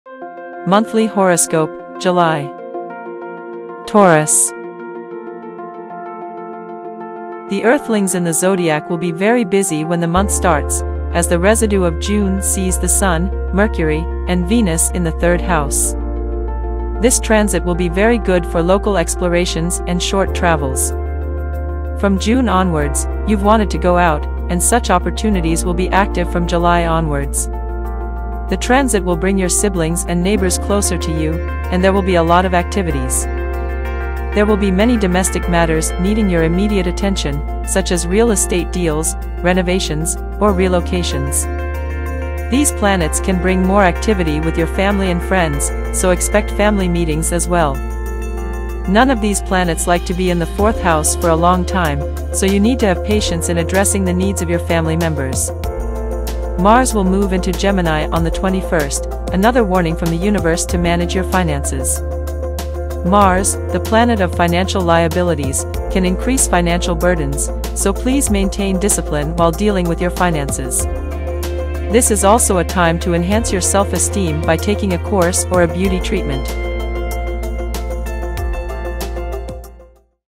Taurus July 2024 Horoscope | Monthly Horoscope
July has arrived. Check out our monthly horoscope for July 2024 to find out accurate predictions of your love life, career, and more.